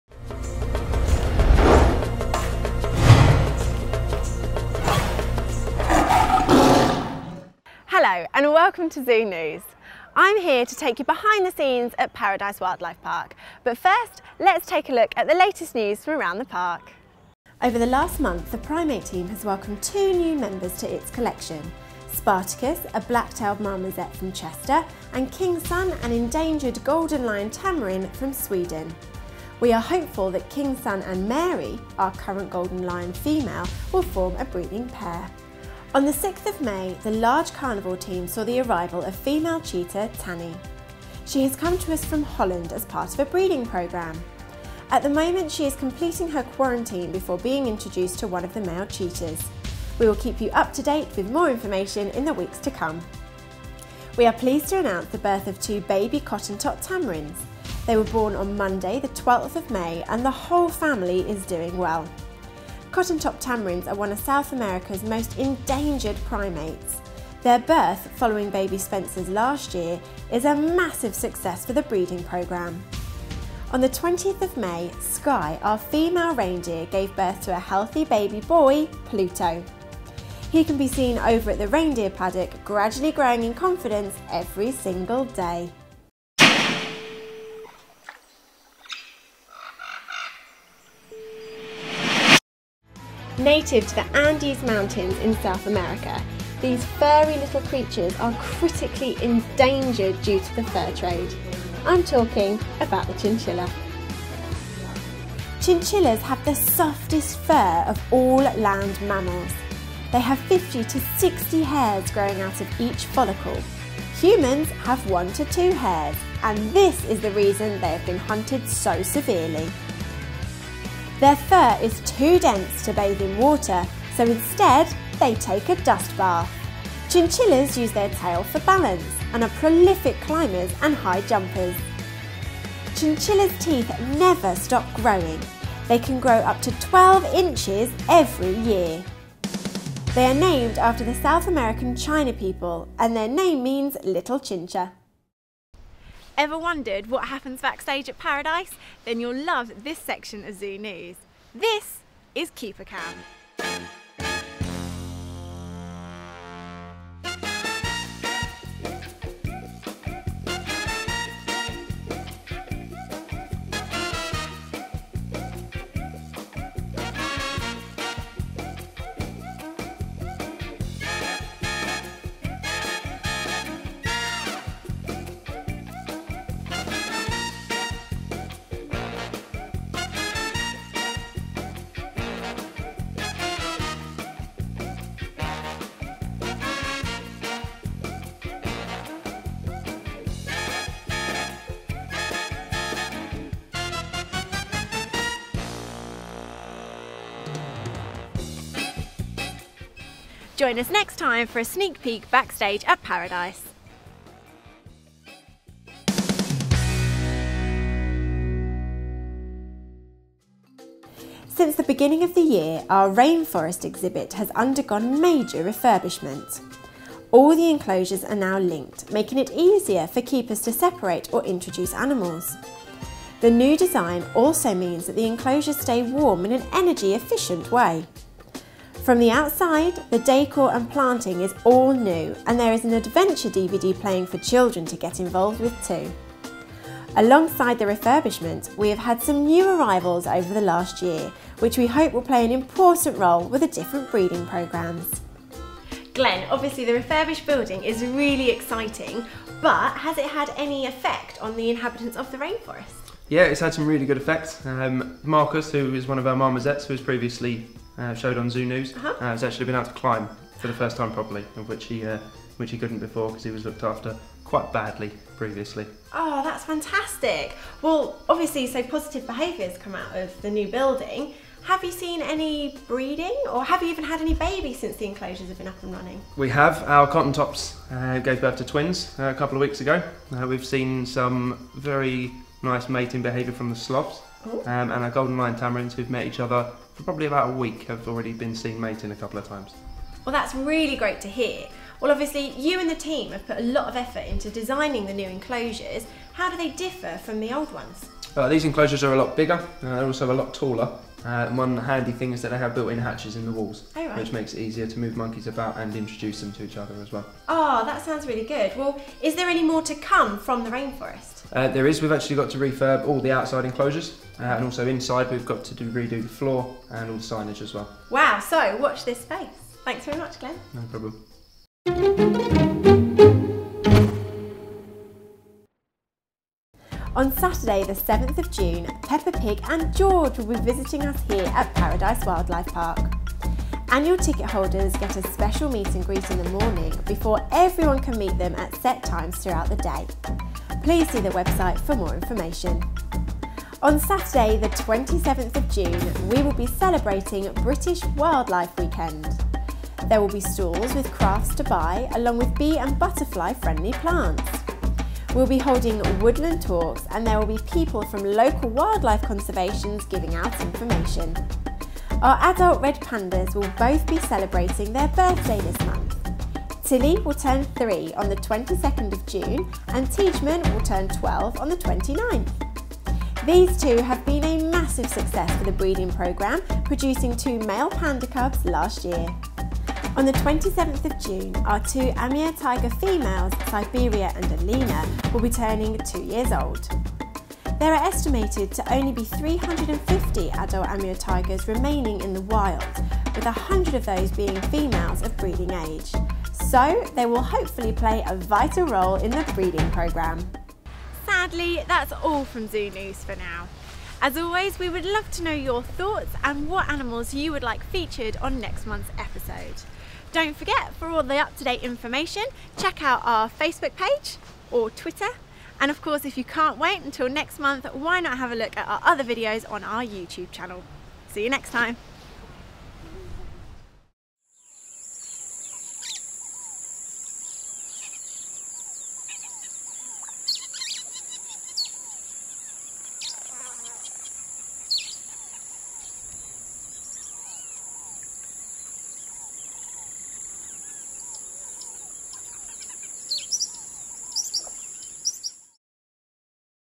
Zoo News | June 2014
As you guys saw last month, we had two Cotton Top Tamarin's born here at the park. We are now pleased to announce the birth of a REINDEER CALF! His name is Pluto and he was born on the 20th May!

To see exclusive footage of Pluto, this months Keeper Cam, our new Rainforest exhibit, amongst various other awesome things from Paradise, check out this months Zoo News!!